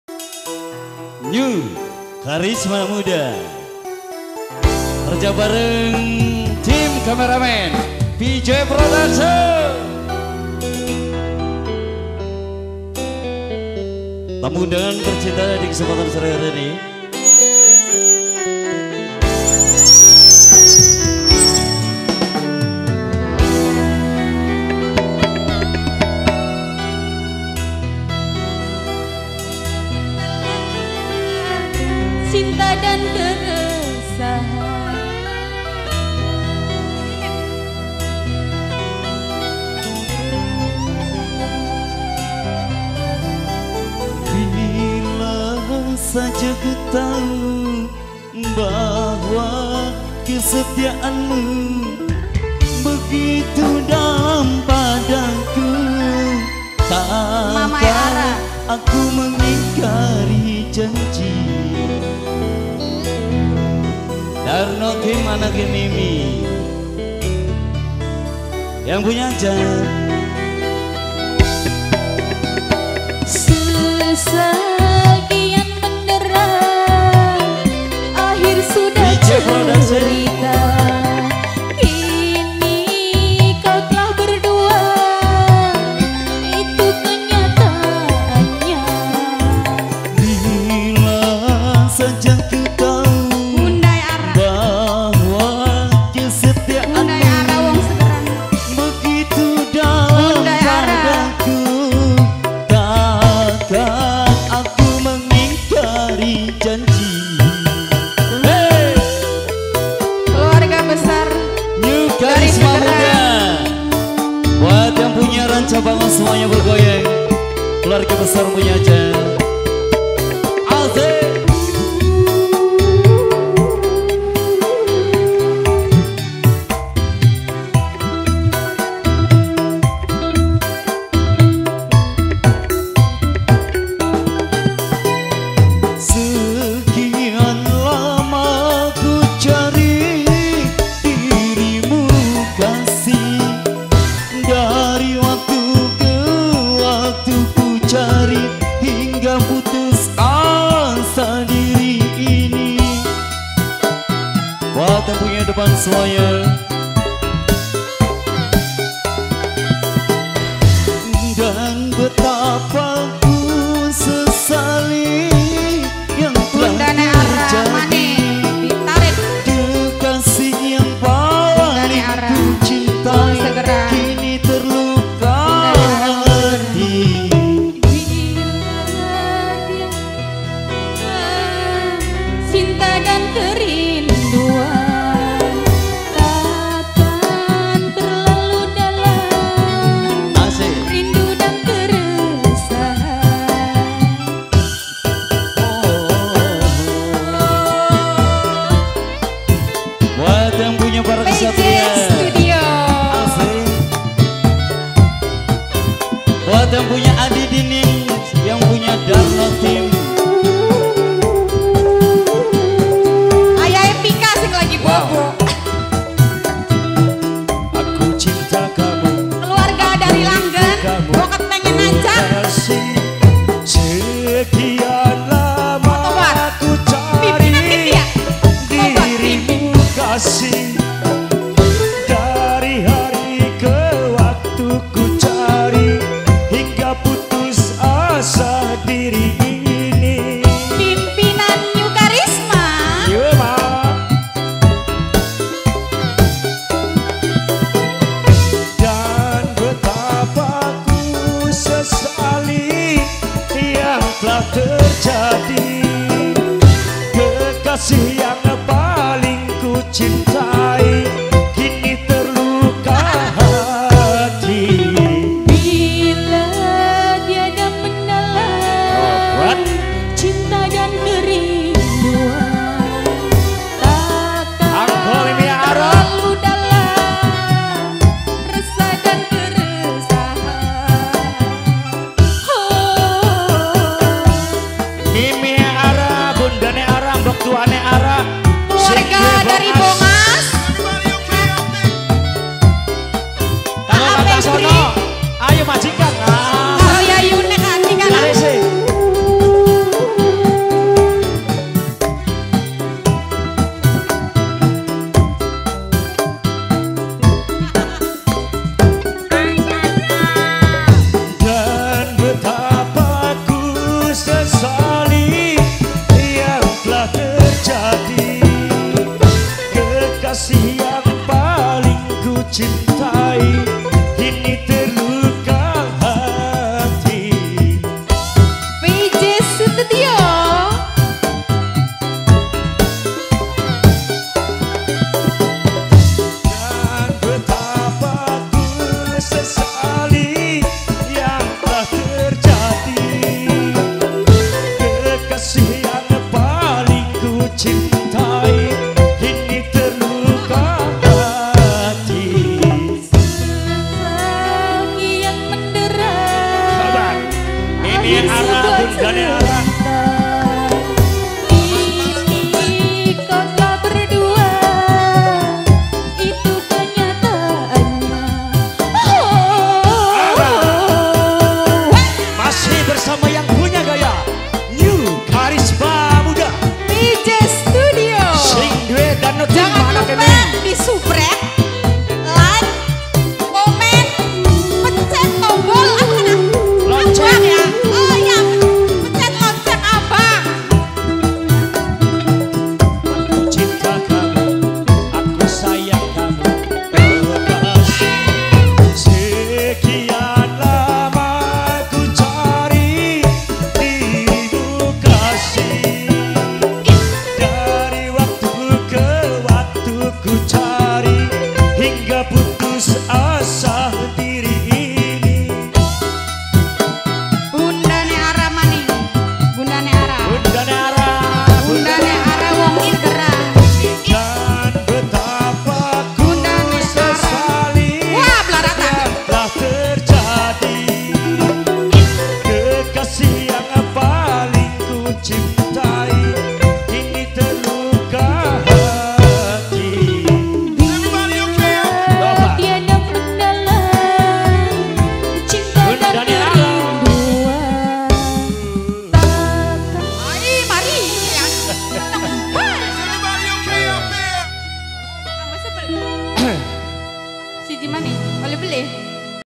KANDAS VOC. BUNDAE FHIKA FT NEW KHARISMA MUDA 27-10-2021 LIVE SUBANG
PESTA HAJATNYA BPK.TASONO - IBU.WULAN
DALAM RANGKA RASULAN : RAHMA ERNA WATI - TIARA NOVITA SARI
DI MERIAHKAN OLEH : NEW KHARISMA MUDA PIM AA DIDIN HP : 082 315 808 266
DOKUMENTASI OLEH : VJ.STUDIO PRO BONGAS : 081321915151